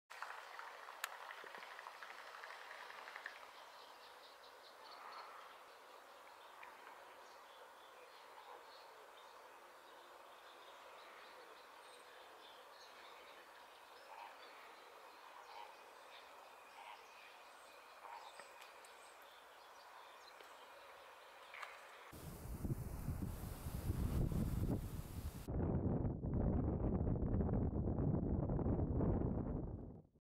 Malvasías volando lento
Ahora se pueden observar las peleas de los machos de Malvasía Cabeciblanca en plena época de reproducción, peculiar pato buceador que estuvo a punto de extinguirse en la década de los 70¨s donde sobrevivía en pequeñas lagunas del Sur de Córdoba